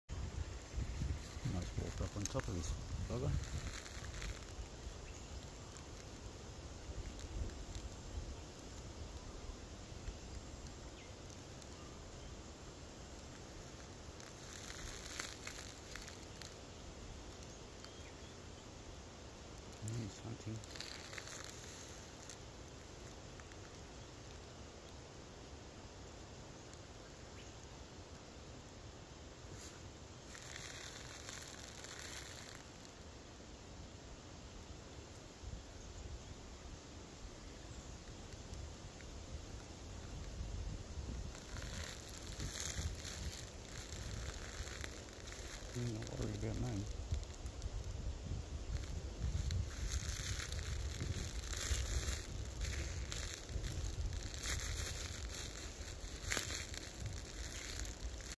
Red belly black snake searching for food.
I found this snake when I was looking for another spot for my trail cam. I was only wearing thongs and shorts so I wasn't really keen on letting it come too close.